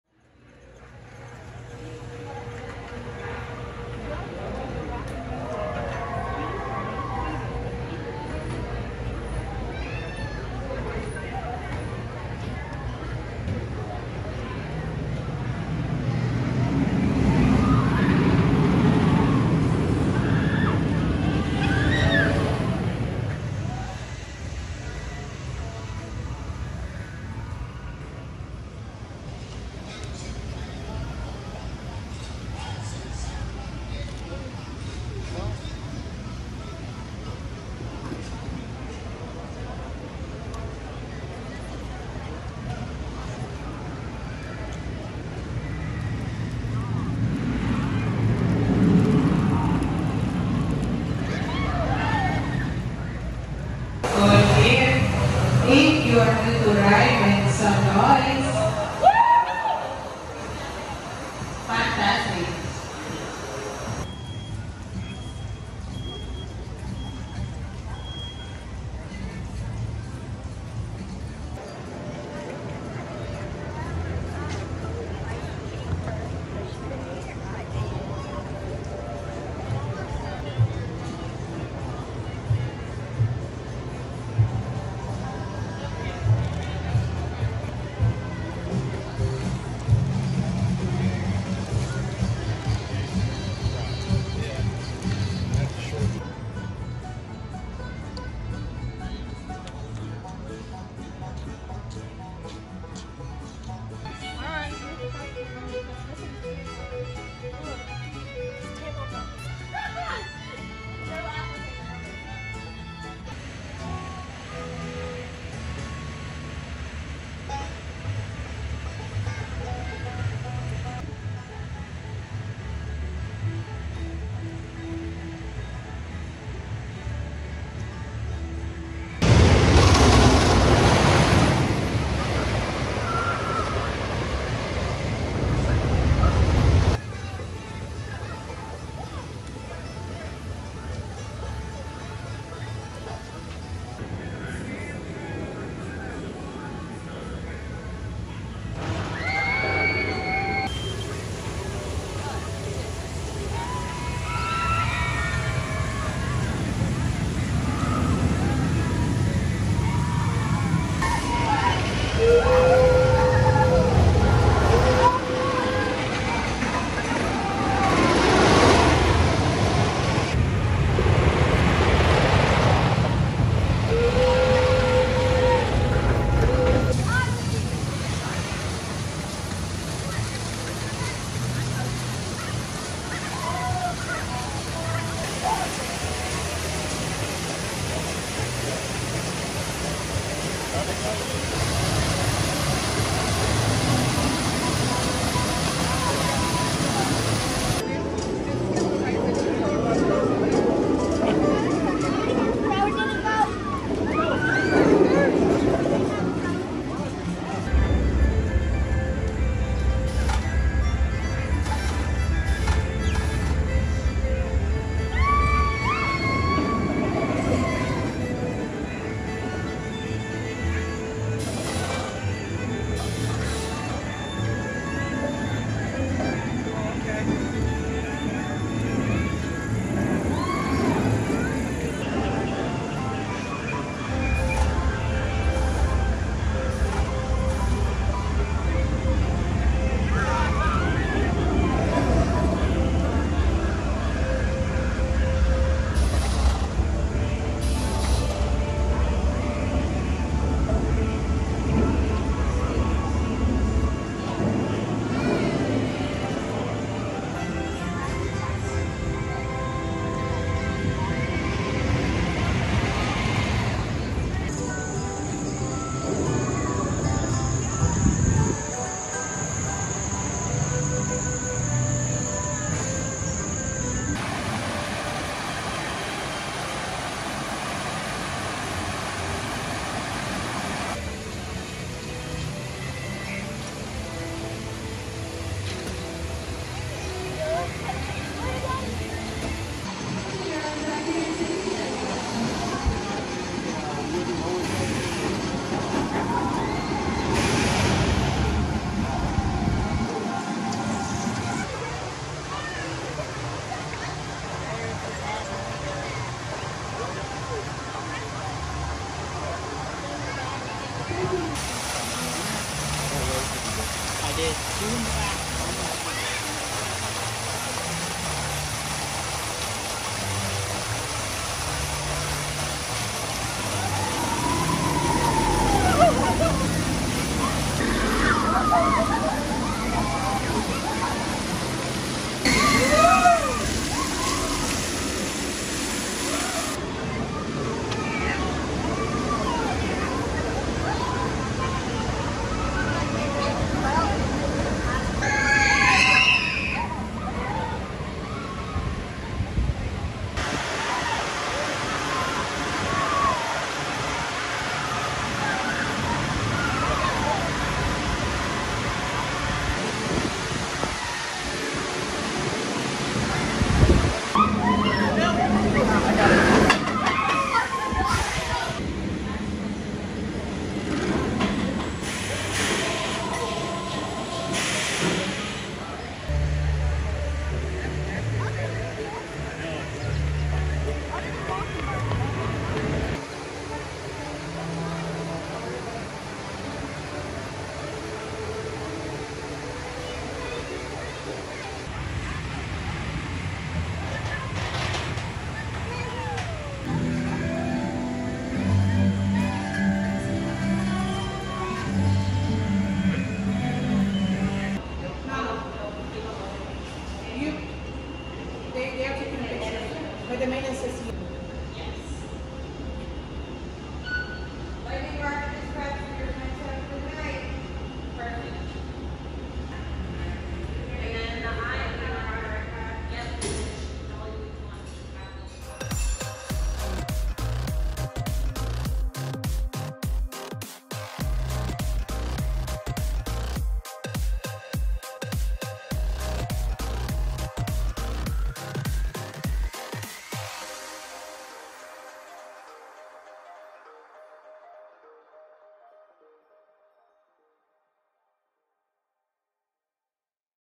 Dollywood Theme Park 4K Footage
Check out a summer day at Dollywood with this NO COPYRIGHT footage!  This world class park is always a good time home to some of the best coasters out there!